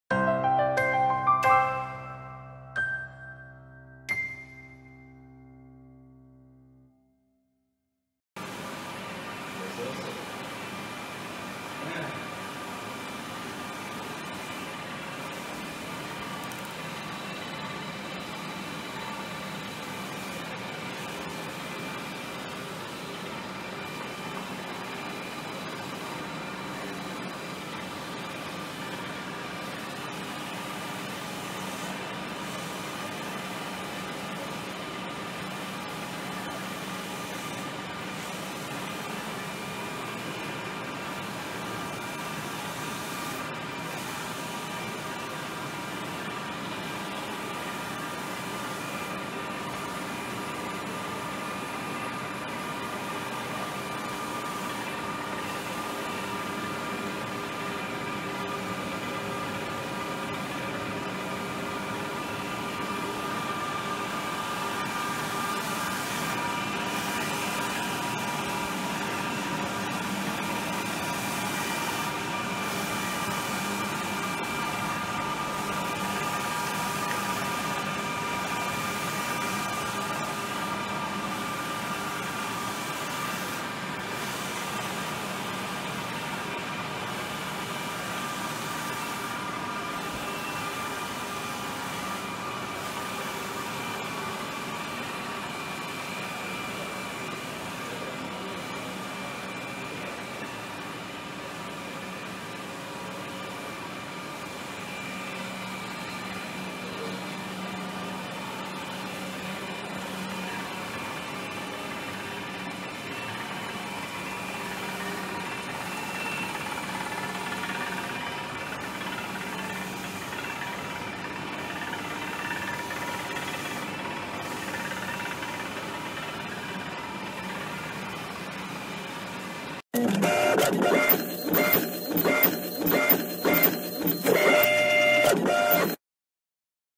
OnePrint Epson L1440 Head UV Roll to Roll Printer/ Plotter GT-3200 series for Soft Film Printing
Whether you’re just starting out or wanting to expand production, the GT-3204 is your ideal print solution for all signs, banners, wallpapers,  labels, and stickers. 3.2m printing width allows different sized materials, as well as Epson L1440 industrial grade printhead is suitable for high volume production. The top configuration helps you run business easily and quickly.